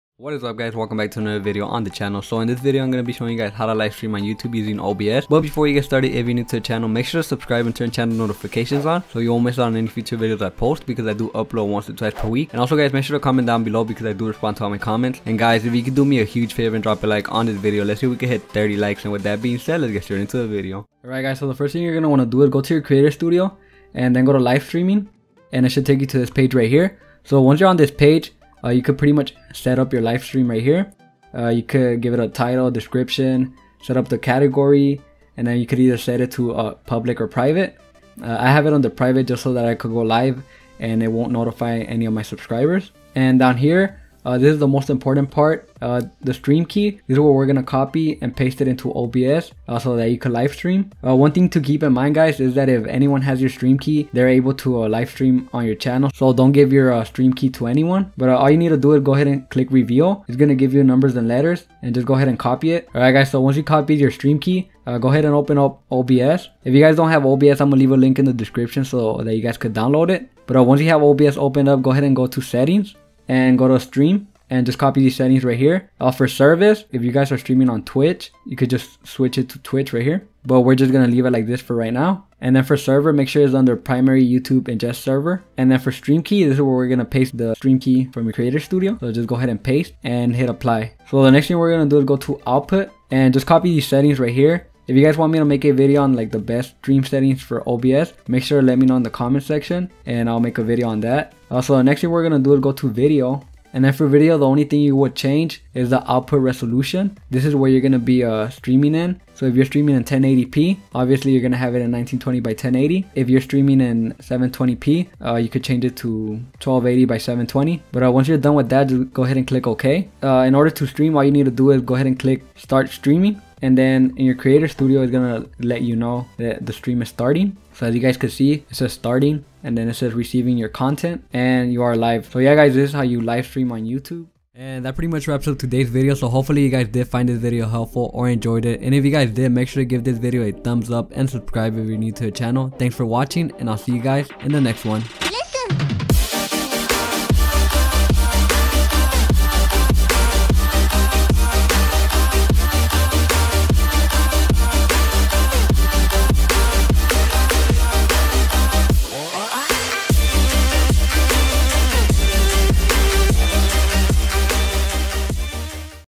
How To Live Stream On YouTube! NEW 2017
Hey Guys!In This Video Tutorial Ill Be Showing You How To Live Stream On Youtube! In This Video Ill Be Showing You How To Live Stream On Youtube Using Obs And The Best Settings For Obs. Live Streaming On Youtube Is Actually Pretty Simple All You Need To Do Is Setup Your Encoder.

:D

MY EQUIPMENT:

Benq gaming monitor:

dell ultrasharp monitor

alienware laptop:

 if You Made It This Far In The Description You Are Awesome!
   Know You Made It This Far In The Description;)